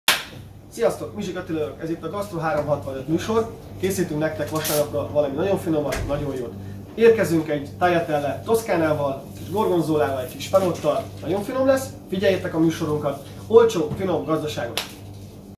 Gasztro365: indul a házi főzőshow!
Extravagáns fogások, pici pénzből, az otthon betárazott alapanyagokból? Vasárnap délelőtt indul a Gasztro365, a Szeged365.hu házi videoblogja, egyben gasztro-showja, benne hétről hétre segítünk, hogy az önkéntes karantén idején is Te lehess a konyha tündére... mit tündére, Istennője!

A Gasztro365 főhőse Szeged okleveles gasztro-alkoholistája, a Pizzamonster tulaja, a Konyhafőnök egykori versenyzője, a sommelier-ből lett séf, Mizsik Attila!